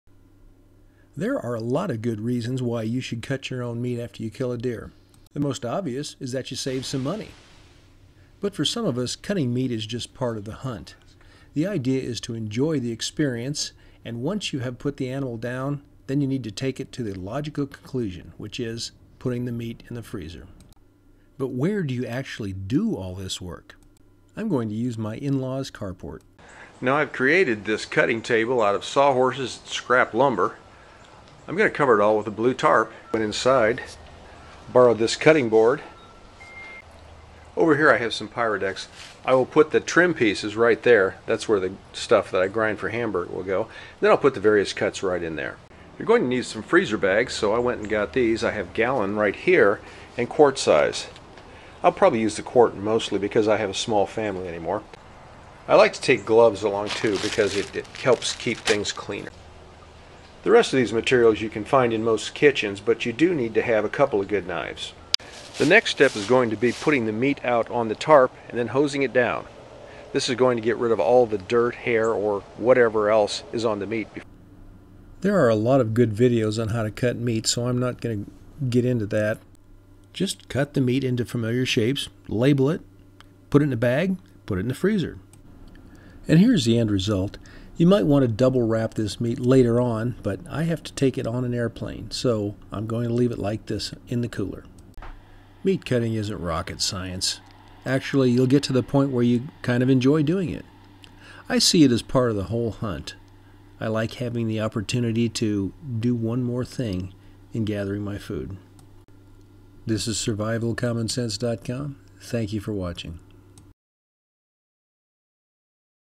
Try this easy at home deer processing setup
Cutting, wrapping and freezing deer meat is not hard and can be learned quickly. But where do you do this meatcutting? Here's how to make a simple setup that will work just fine for most of your meatcutting needs.